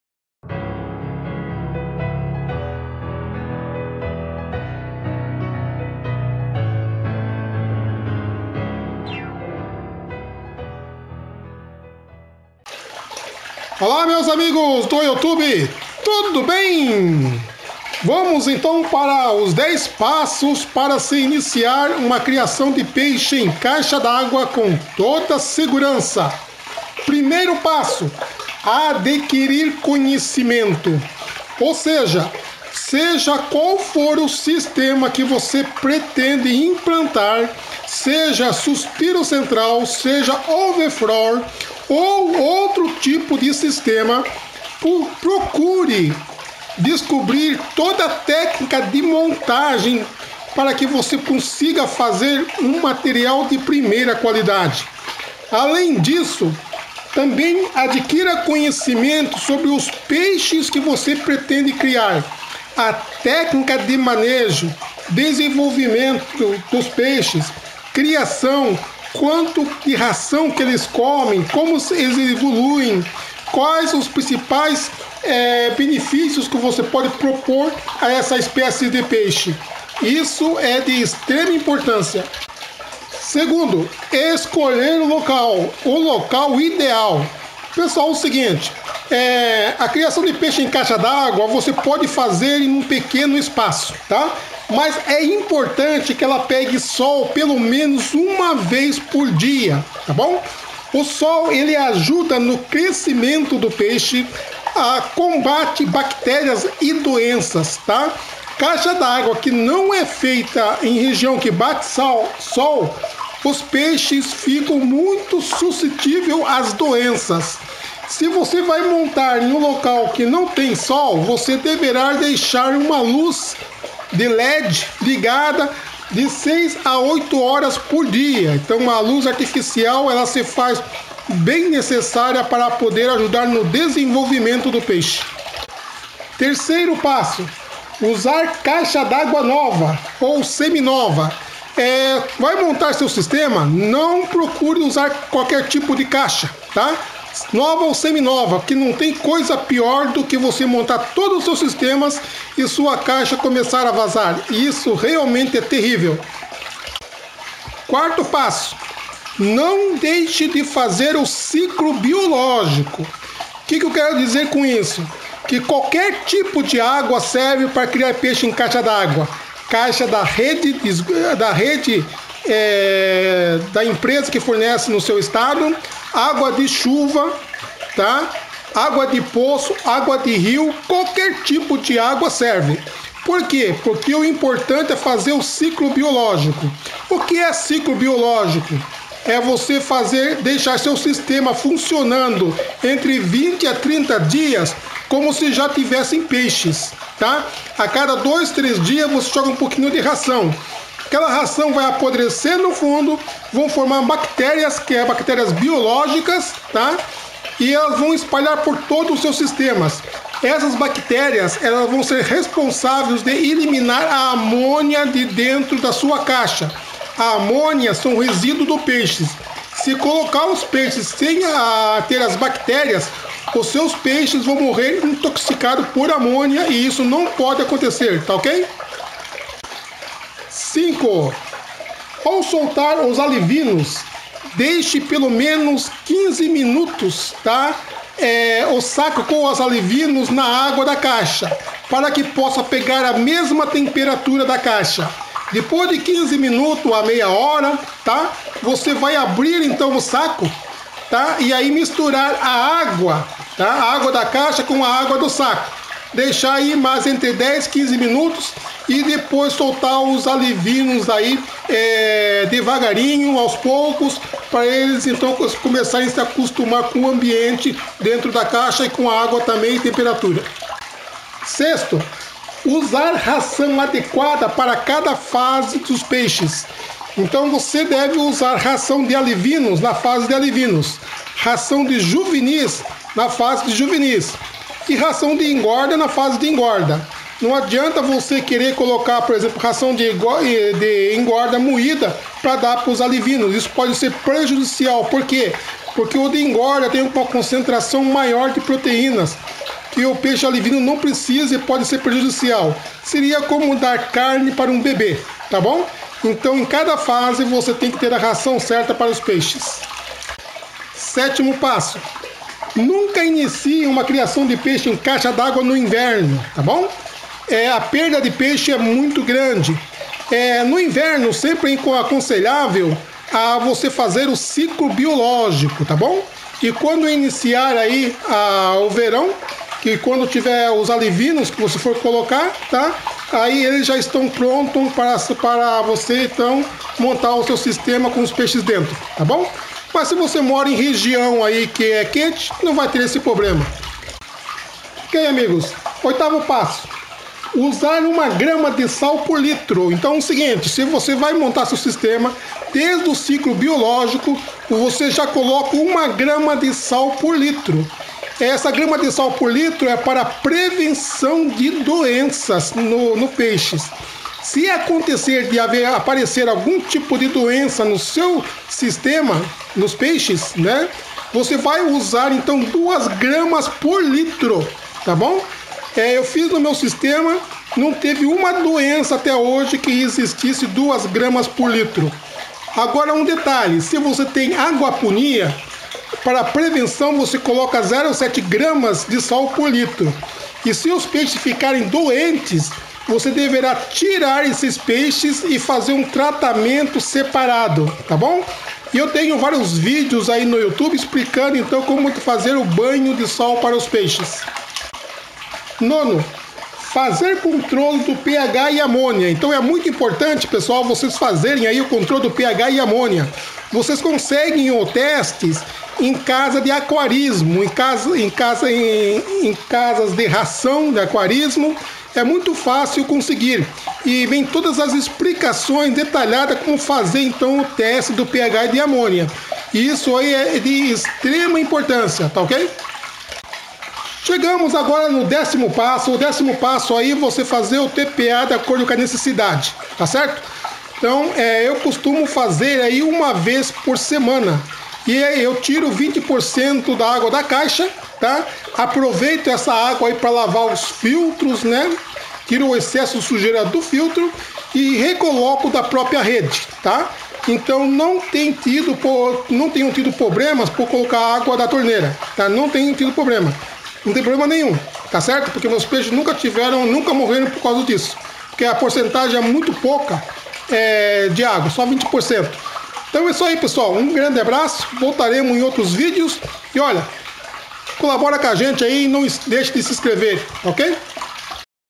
DEZ PASSOS PARA SE INICIAR UMA CRIAÇÃO DE PEIXES EM CAIXA D`ÁGUA COM SEGURANÇA .
INVISTA EM SEU SONHO. VAMOS FAZER ACOMPANHAMENTO DESDE A MONTAGEM DA CAIXA AO ABATE DOS PEIXES. ADQUIRA O CURSO EM ATÉ 12 VEZES: LINK PARA COMPRA